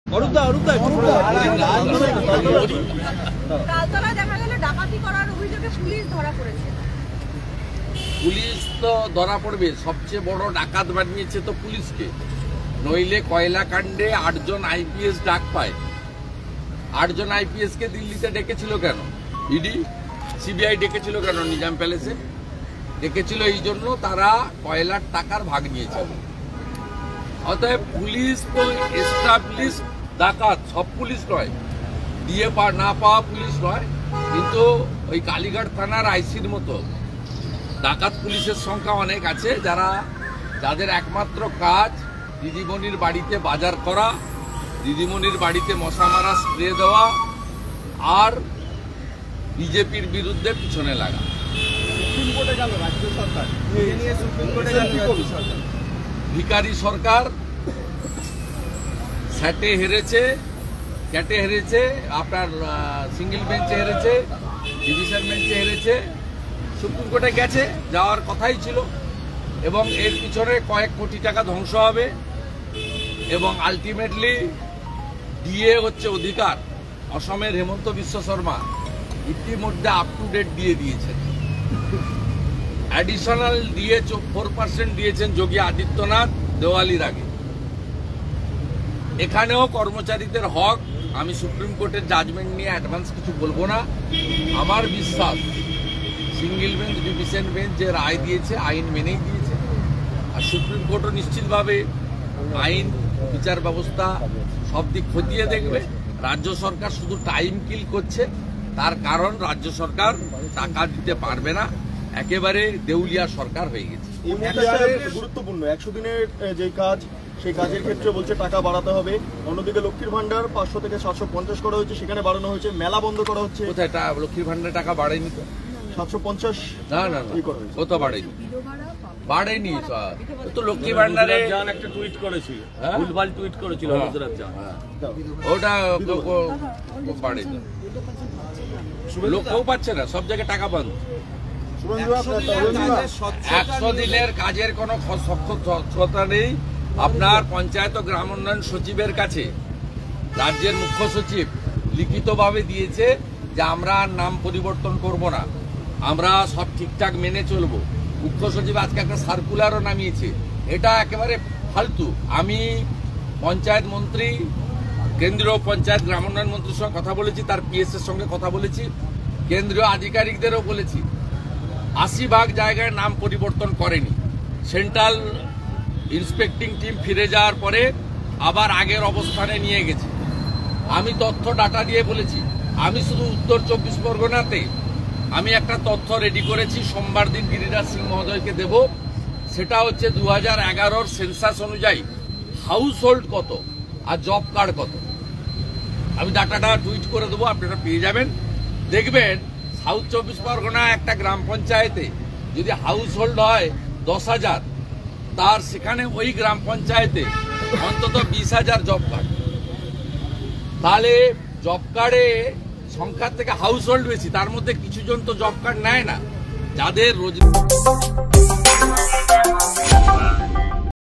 'ডাকাত পুলিশের কাজ দিদিমনির বাড়িতে বাজার করা', বিস্ফোরক শুভেন্দু
কলকাতা টুডে ব্যুরো: কলকাতা পুলিশকে ডাকাত বলে ভৎসনা শুভেন্দু অধিকারীর। বিধানসভার বাইরে প্রকাশ্য রাস্তায় পুলিশকে কড়া ভাষায় ভৎসনা করলেন বিরোধী দলনেতা শুভেন্দু। তিনি বলেন,"সবচেয়ে বড় ডাকাত বানিয়েছে পুলিশকে নইলে কয়লা কাণ্ডে ৮ জন IPS ডাক পায়? মিলে কয়লাকান্দে ৮ জন IPS ডাক পায়? ৮ জন IPS-কে CBI ED ডেকেছিল কেন নিজাম প্যালেসে? ডেকেছিল এই জন্য কারণ তারা কয়লার টাকার ভাগ নিয়েছে। অতএব পুলিশ হল এস্টাবলিস্ট ডাকাত। তবে সব পুলিশ নয়। DA না পাওয়া পুলিশ নয়। তবে ওই কালিঘাট থানার IC-এর মত ডাকাত পুলিশের সংখ্যা অনেক আছে। যাদের একমাত্র কাজ দিদিমনির বাড়িতে বাজার করা, দিদিমনির বাড়িতে মশা মারার স্প্রে দেওয়া আর বিজেপির পেছনে লাগা"

শুভেন্দু বকেয়া DA প্রসঙ্গ তুলে বলেন,"ভিখারি সরকার! বিভিন্ন জায়গায় হেরেছে। সিঙ্গেল বেঞ্চে হেরেছে ডিভিশন বেঞ্চে হেরেছে। সুপ্রিম কোর্টে গেছে যাওয়ার কথা ছিল। এবং এর পেছনে কয়েক কোটি টাকা ধ্বংস হবে শেষ পর্যন্ত। অসমের হেমন্ত বিশ্ব শর্মা ইতিমধ্যে আপটুডেট দিয়ে দিয়েছেন। দিওয়ালির আগে অ্যাডিশনাল DA-এর 4 পার্সেন্ট দিয়েছে যোগী আদিত্যনাথ। এখানেও কর্মচারীদের অধিকার তবে আমি সুপ্রিমকোর্টের রায় নিয়ে কিছু বলবো না আমার বিশ্বাস সিঙ্গেল বেঞ্চ ডিভিশন বেঞ্চ যে রায় দিয়েছে আইন মেনেই দিয়েছে আর সুপ্রিমকোর্ট ও নিশ্চিতভাবেই আইন এবং বিচারব্যবস্থা সবকিছু খতিয়ে দেখে রায় দেবে রাজ্য সরকার শুধু সময় নষ্ট করছে তার কারণ রাজ্য সরকার টাকা দিতে পারবে না একেবারে দেউলিয়া সরকার হয়ে গেছে।"